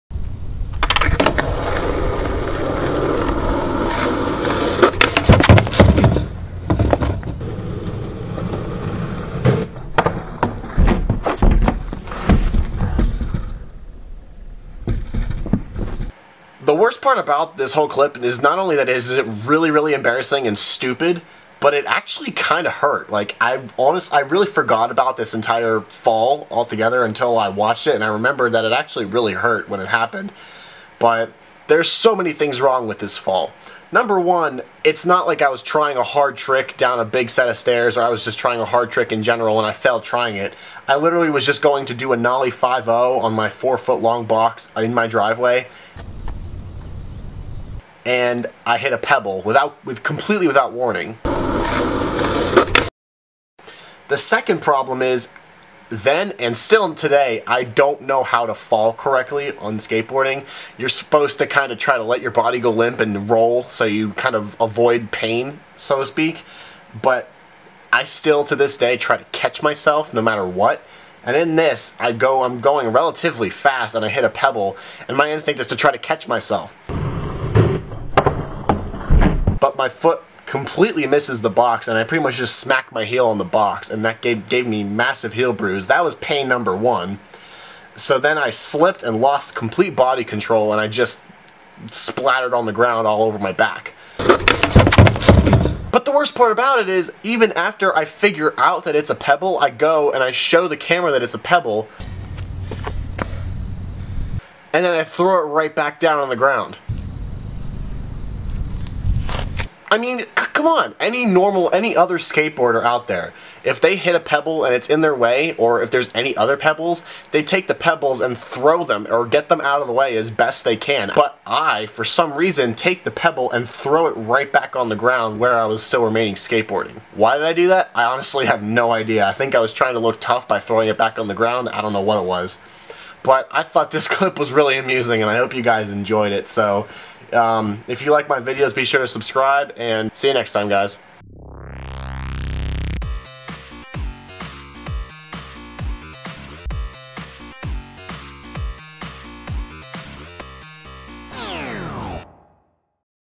HILARIOUS Skater Fall!
I think this might be the funniest and most embarrassing fall I've ever caught on film of myself. Hope you like it!